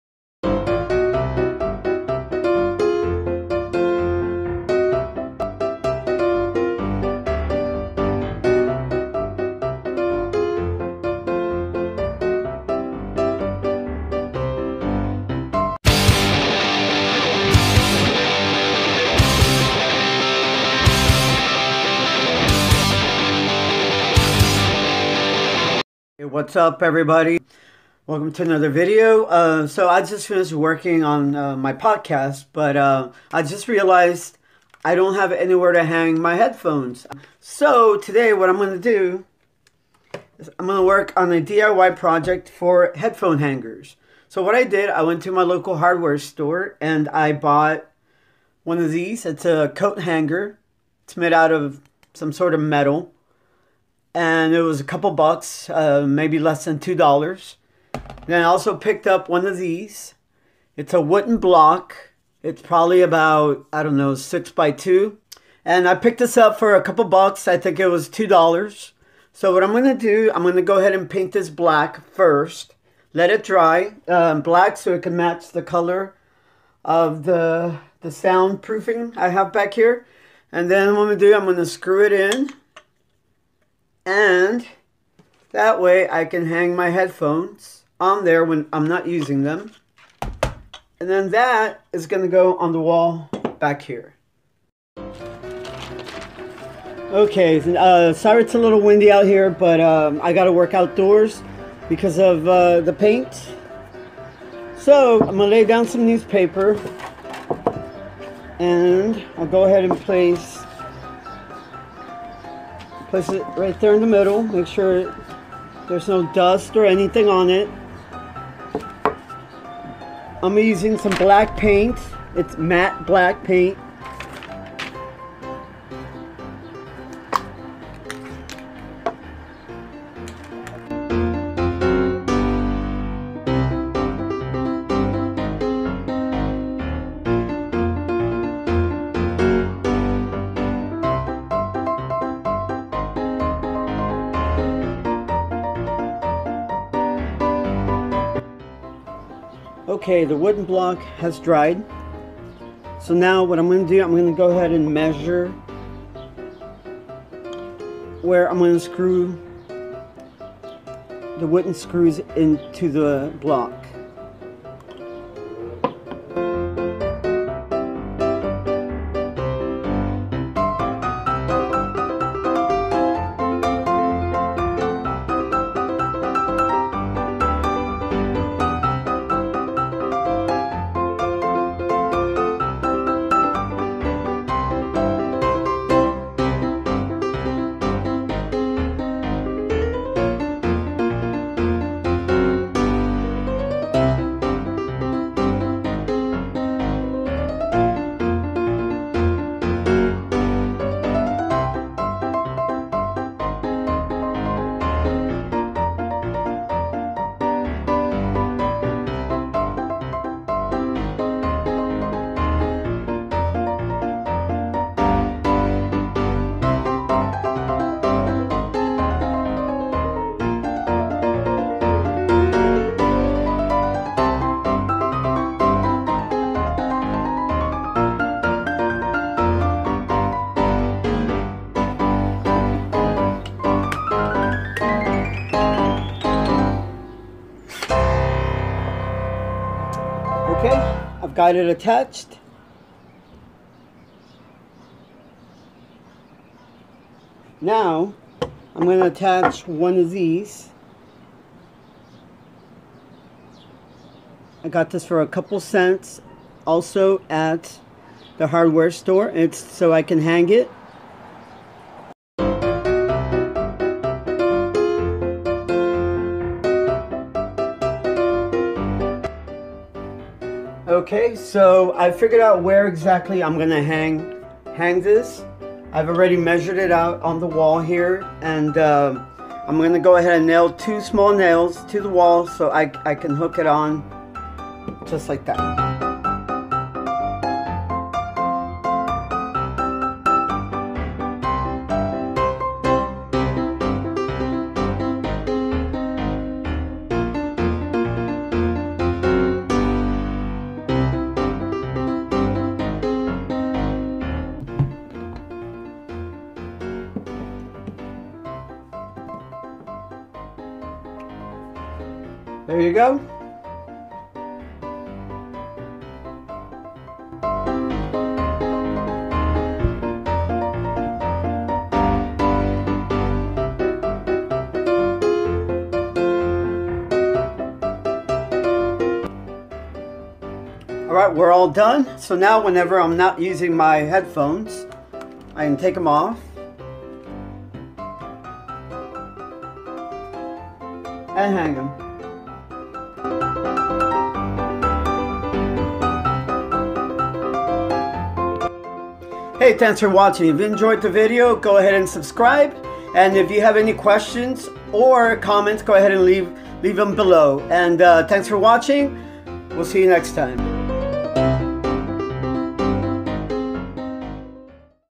Life of Bob: DIY Headphone Hanger project
I clumsly build myself headphone hanger.

Supplies:
1. Metal coat hanger
2. 6x2 wooden block
3. Spray paint
4. Picture frame hanger